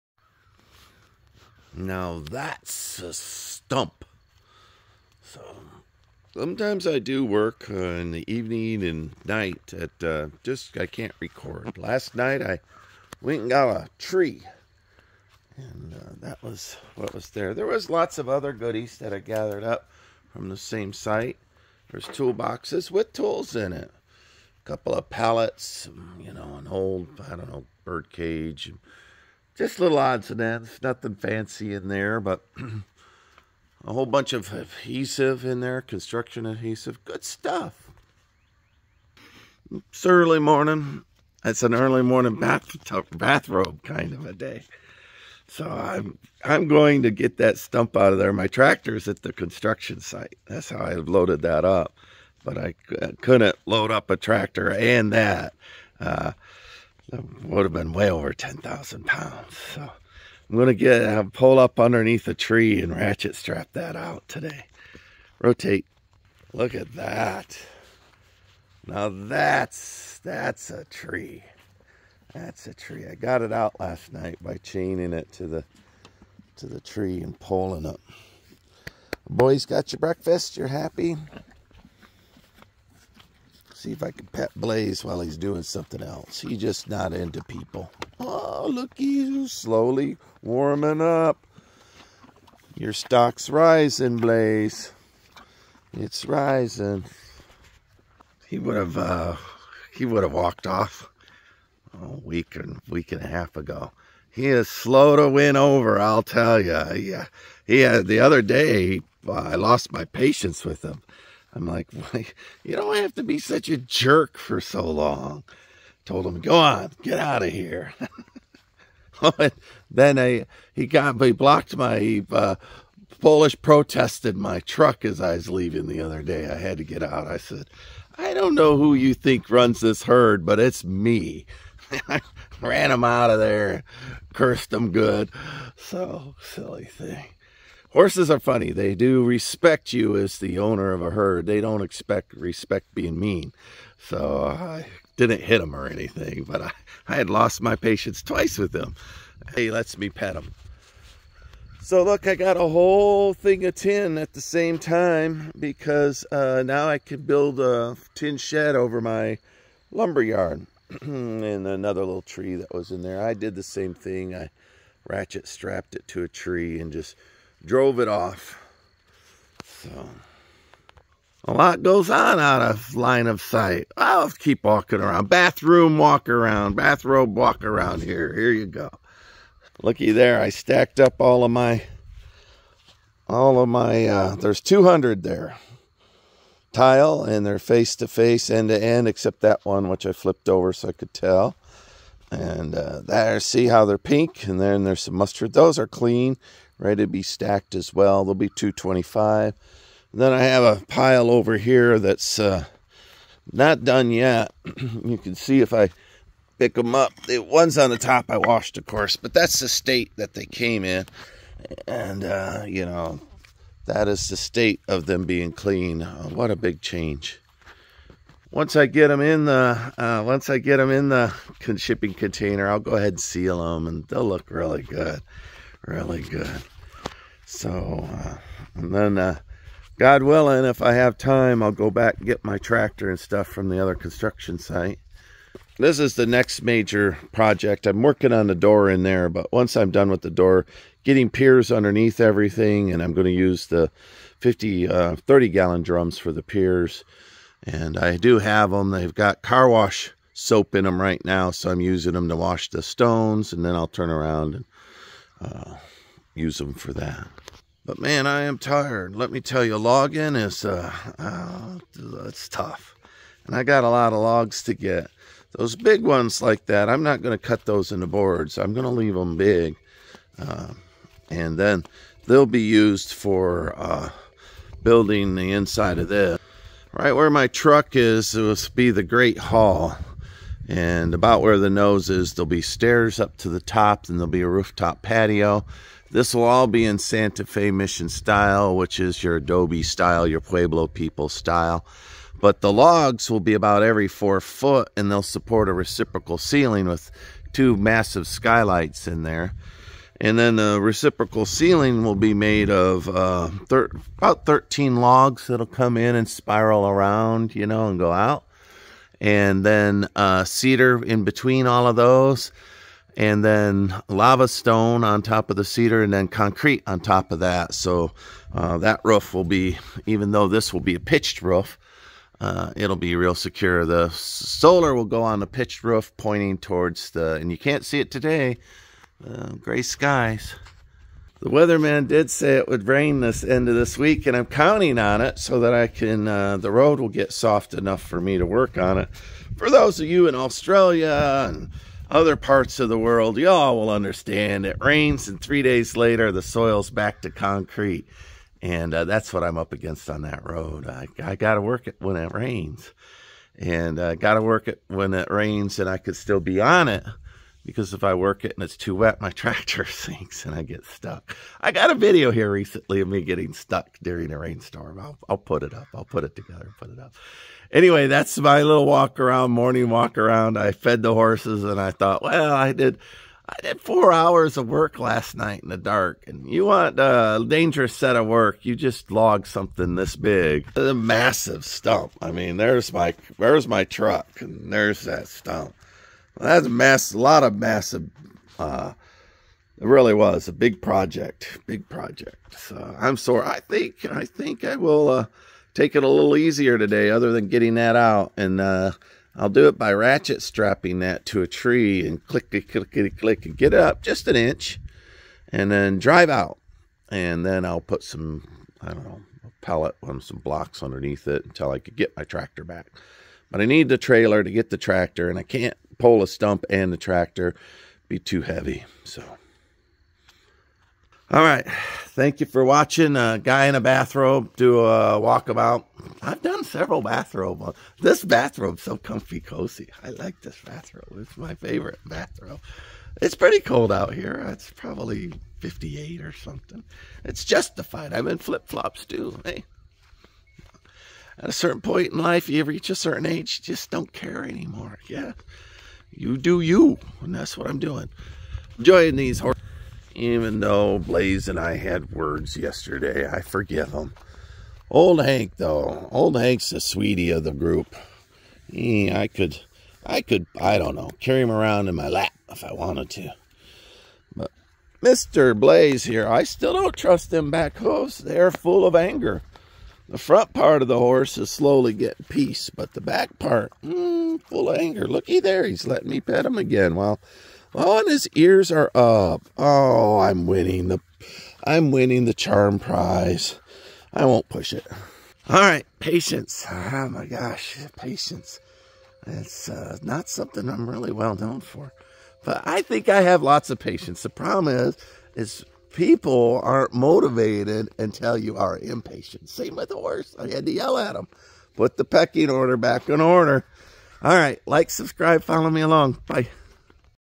Bathrobe walk about of massive log and midnight scrap metal.
Another late night that I wasn't able to record - too dark.

But I harvested a huge cedar as well as collected an entire outbuilding of tin. That will get my OscarPro log mill nicely tucked away.

In morning's light I take a look at what was gather as well as make plans to remove a huge stump by hand from my trailer. The by-hand part seems pretty dicey to me and I will consider my options carefully.